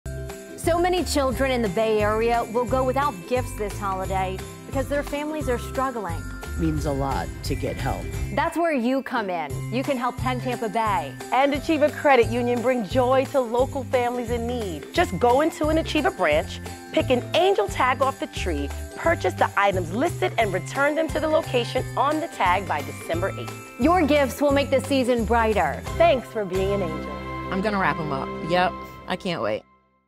Angel Tree - Achieva x 10 Tampa Bay
Achieva Credit Union and 10 Tampa Bay invite you to support local families in need by giving to The Salvation Army's 2023 Angel Tree Program. Donations collected from Achieva branches will help thousands of children across the Tampa Bay and Southwest Florida areas. They will receive clothes, toys, games, and bikes, according to their wish list.

Sponsor an angel today to help them experience an unforgettable holiday season.